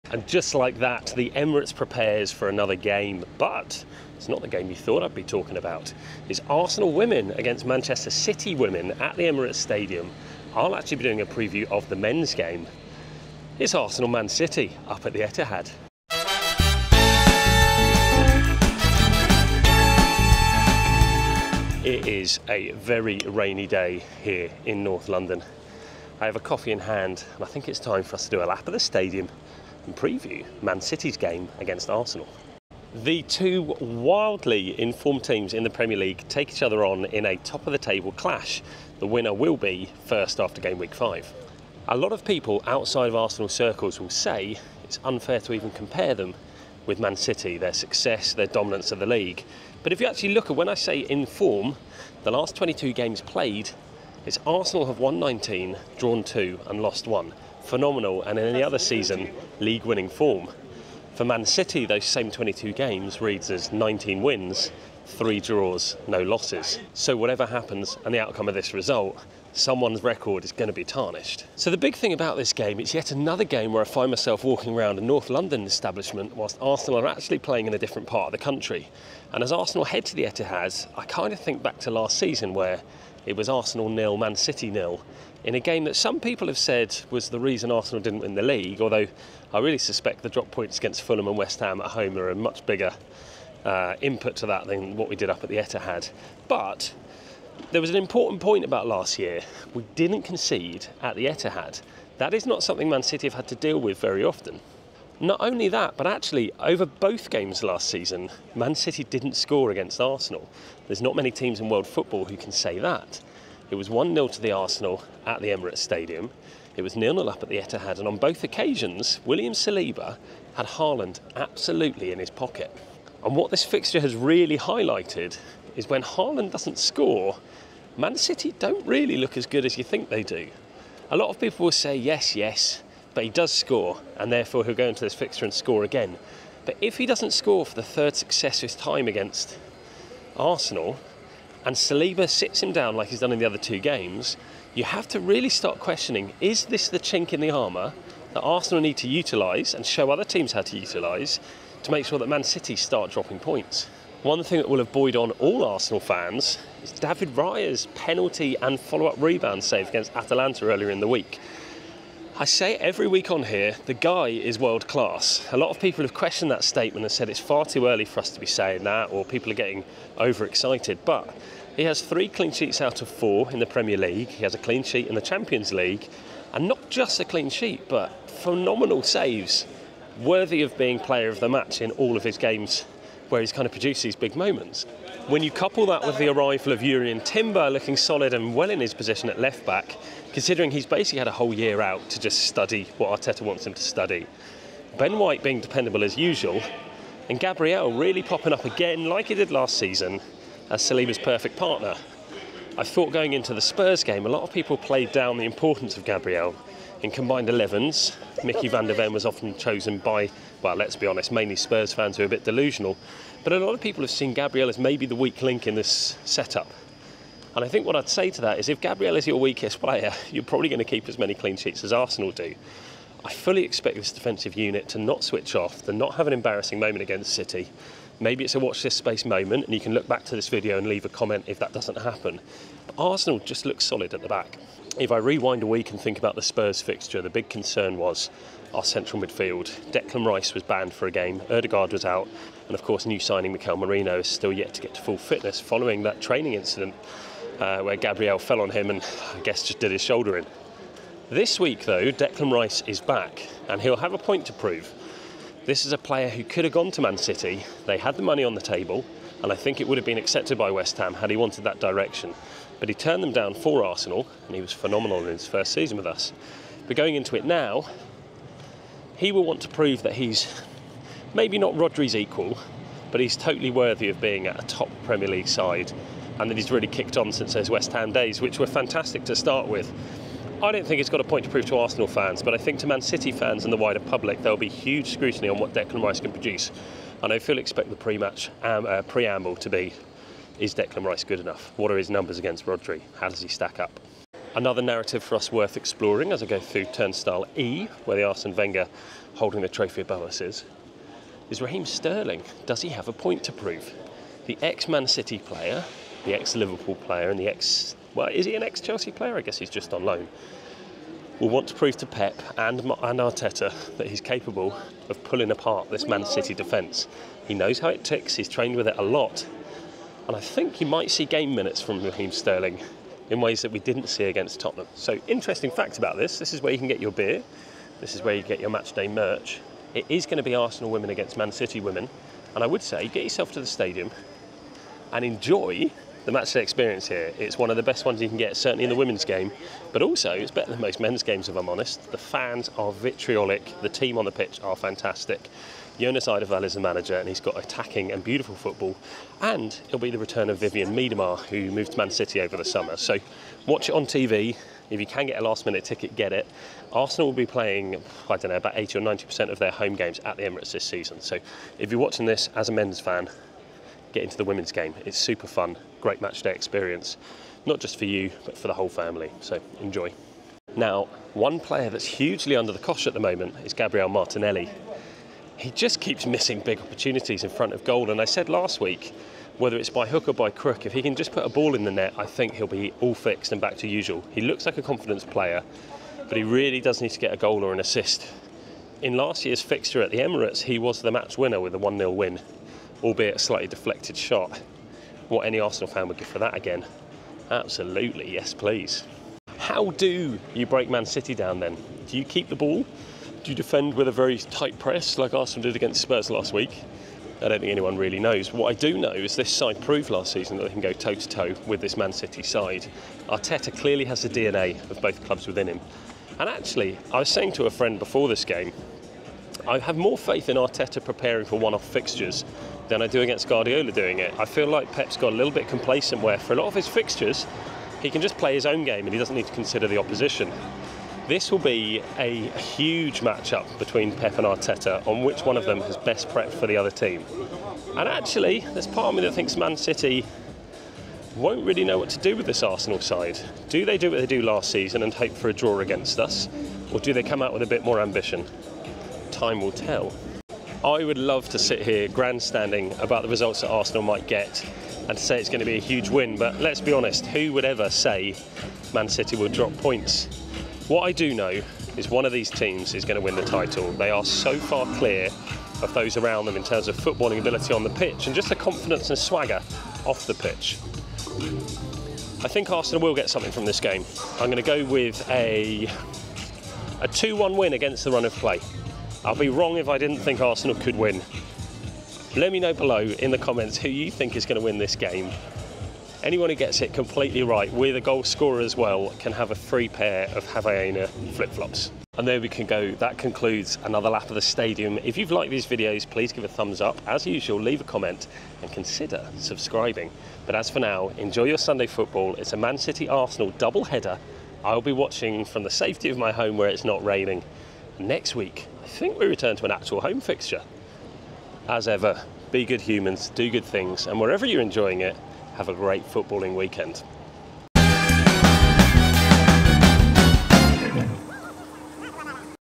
Can Arsenal Beat Man City in the Premier League? | Gameweek 5 | Man City Arsenal Preview
Is this the year we all ask? Arsenal take Man City at the Etihad. Can Arteta turn the tide of will the Orcs of Manchester run riot again? As ever, predictions below...